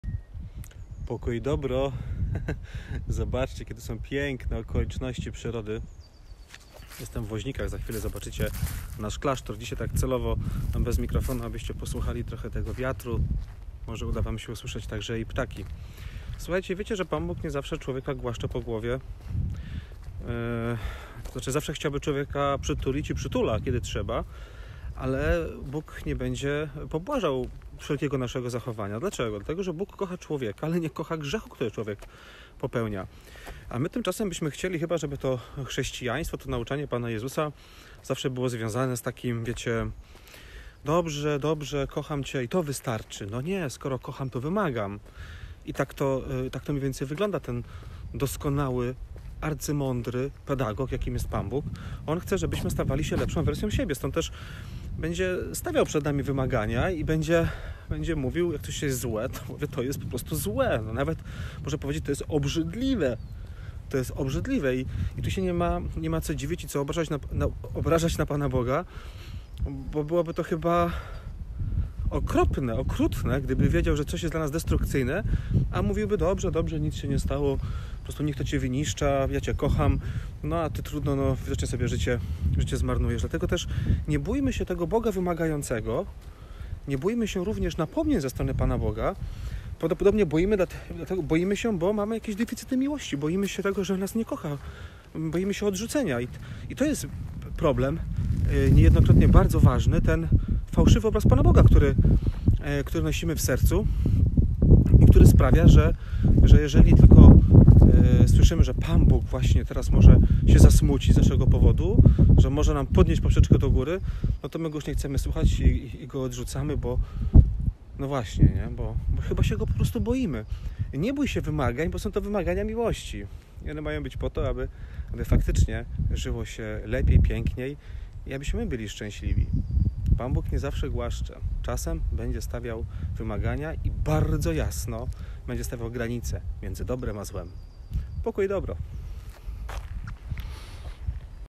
Bóg nie zawsze głaszcze po głowie | dlaczego się Go tak boimy? | offtopic (20)
Jakie musimy mieć deficyty miłości, jaki obraz Boga, skoro częściej się Go boimy niż Mu ufamy. To niestety niesie za sobą poważne konsekwencje. Nie dowierzając w miłość Boga szukamy w Jego oczach akceptacji, nie tylko dla nas samych, ale co gorsze, coraz częściej także dla naszych złych decyzji.